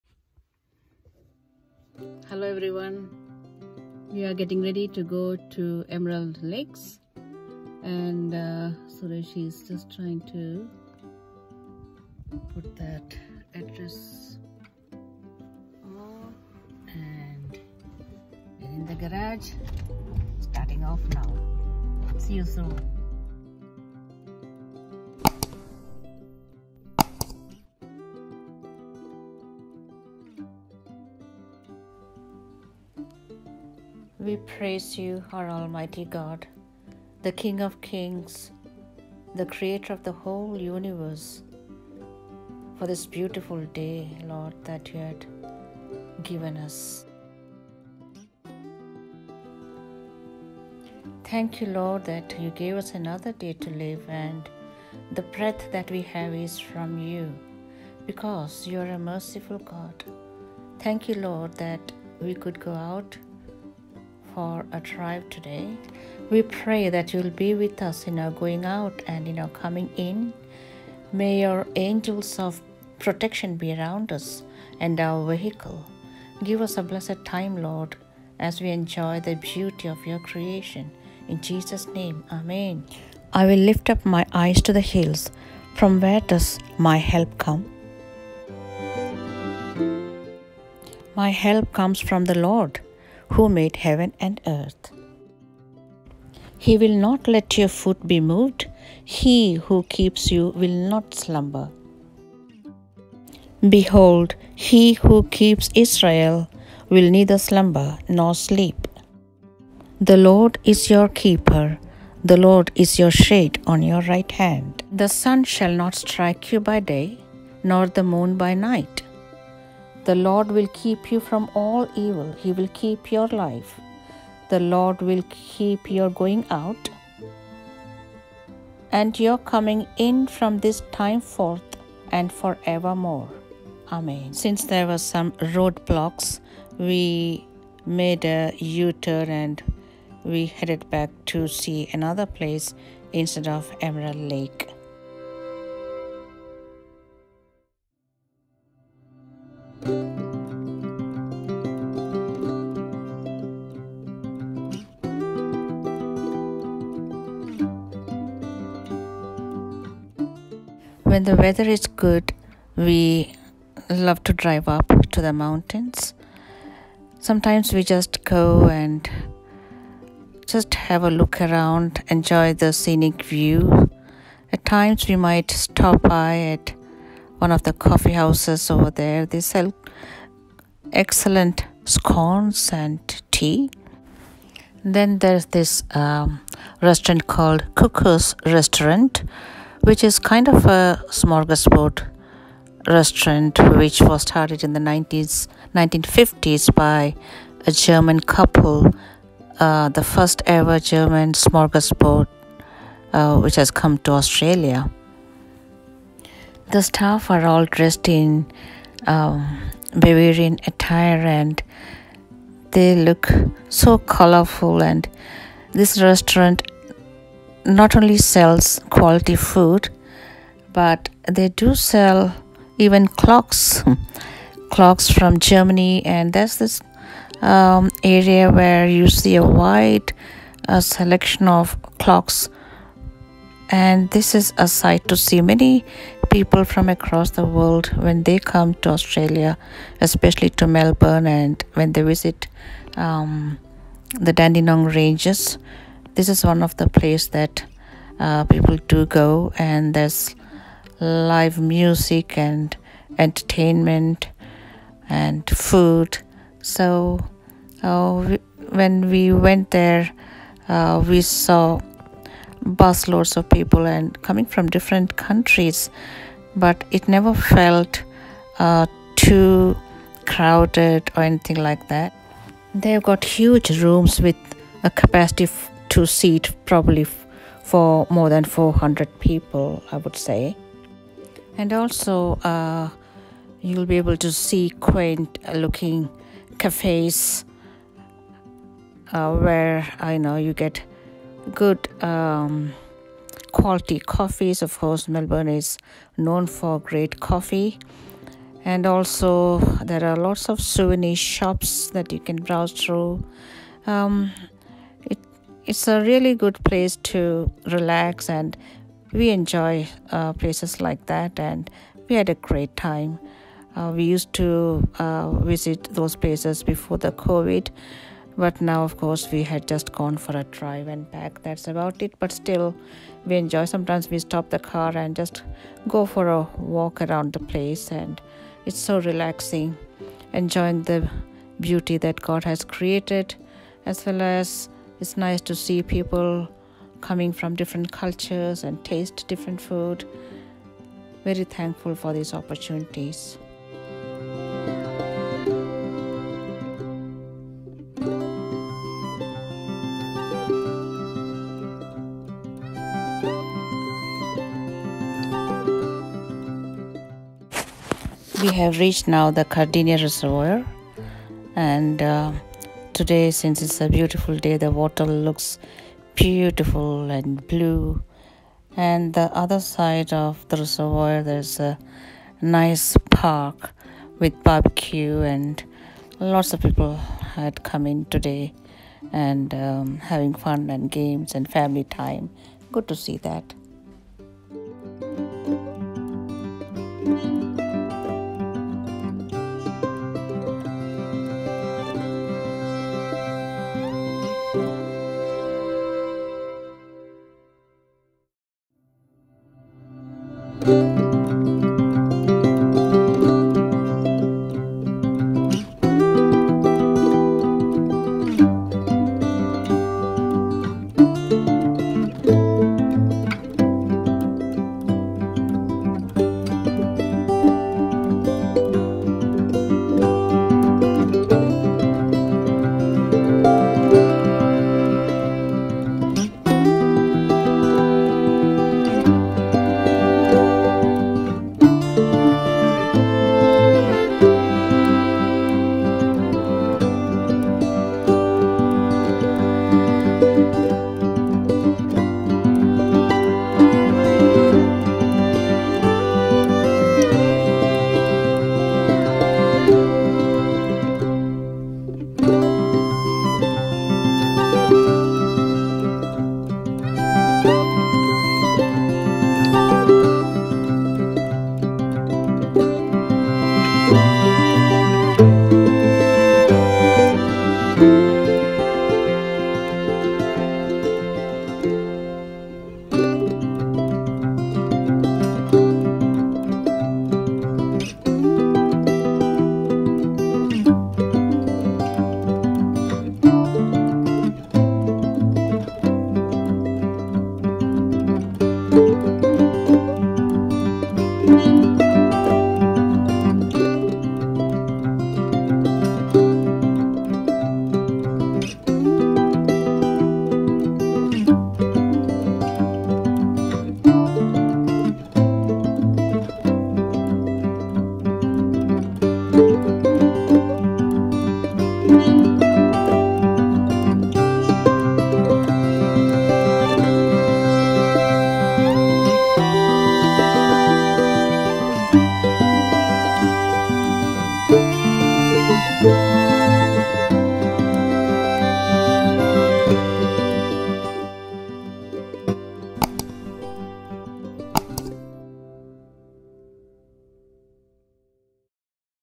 Lakes and Dandenong Ranges
Bible reading from ESV version
Music my Gabriel Witcher

Please watch my friend’s channels which has been a blessing to me.